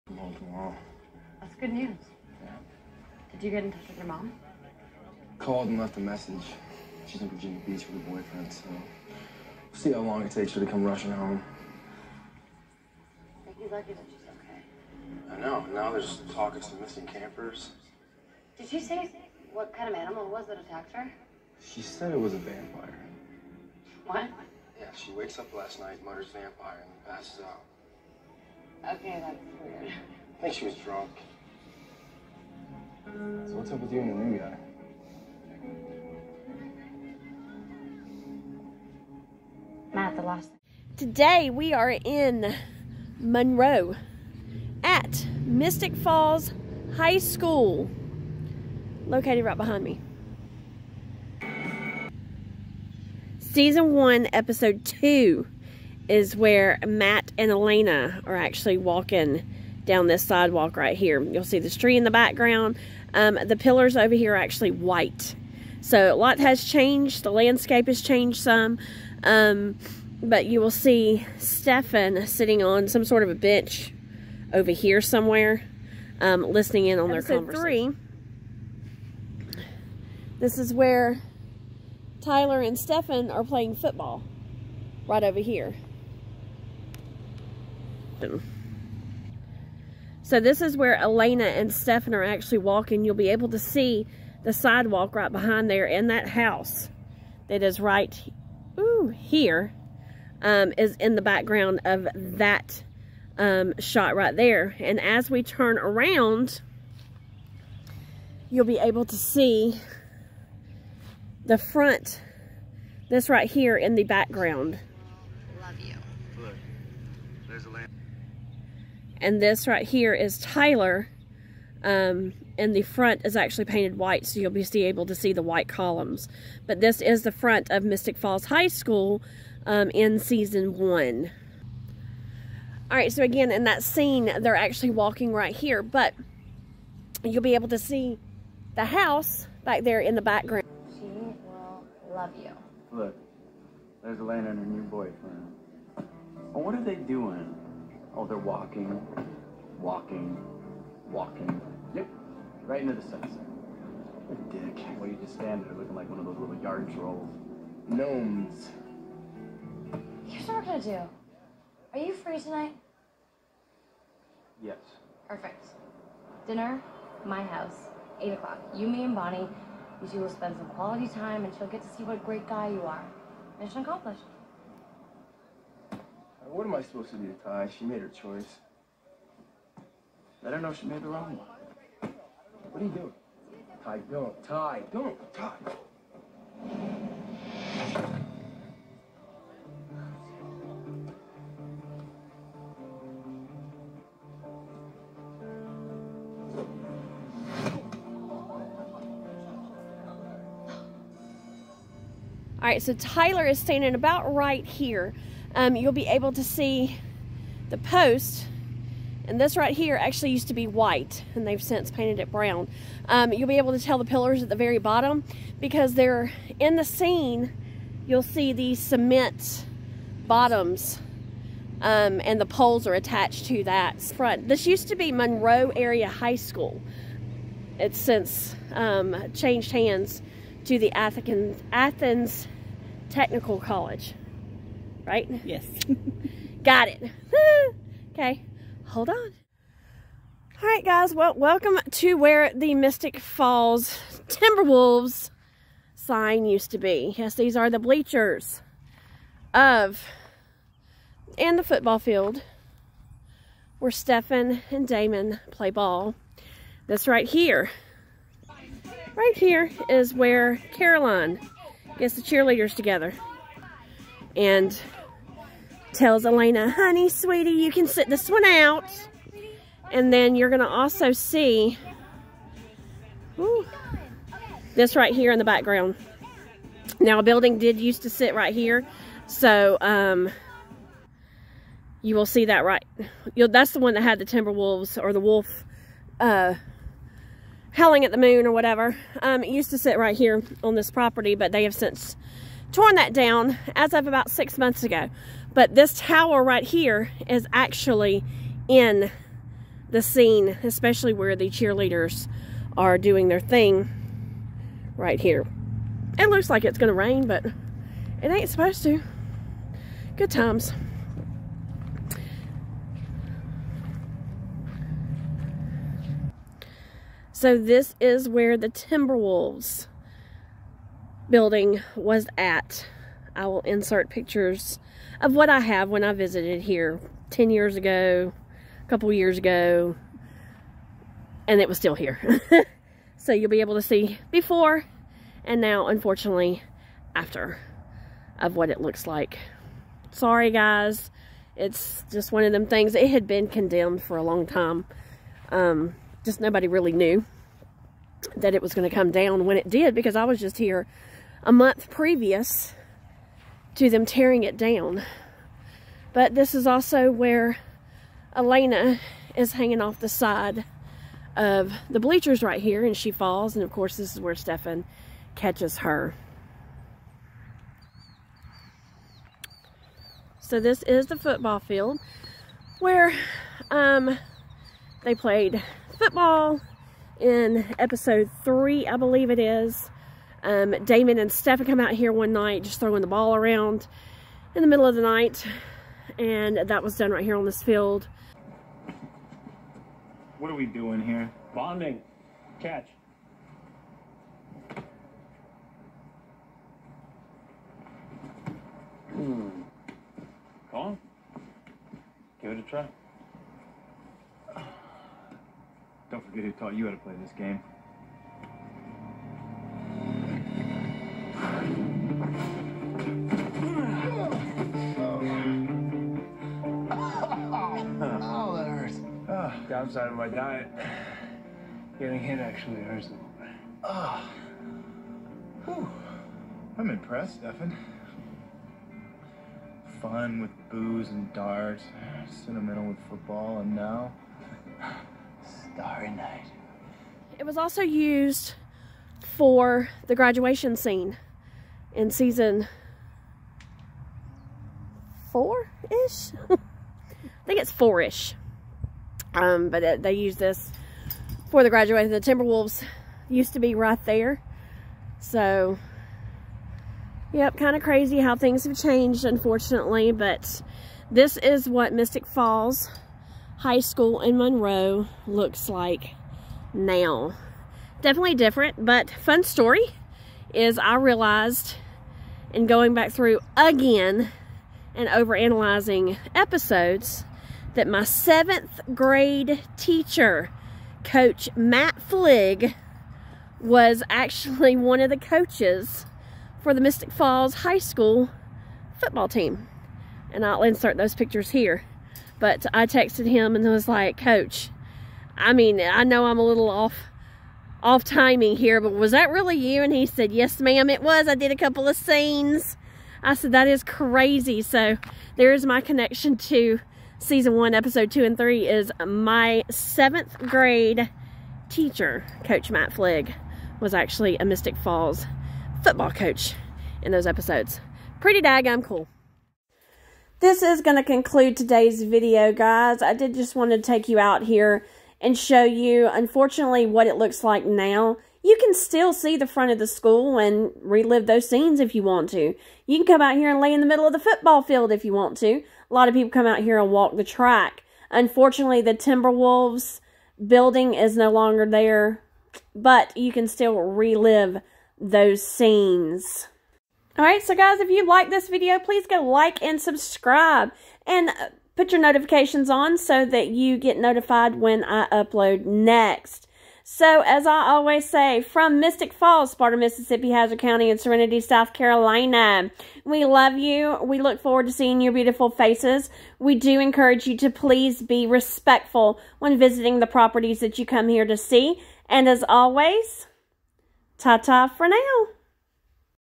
Mystic Falls High School
See the scenes this property was used for. Front of the school and football field.